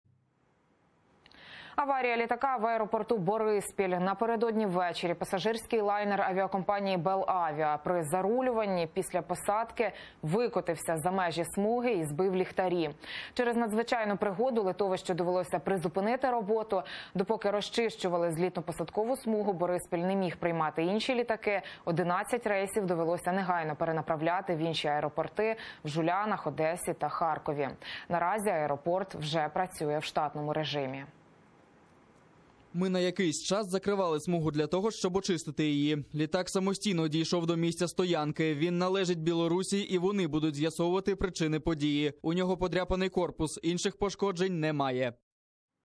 Аварія білоруського літака в «Борисполі». Подробиці
ZAMAN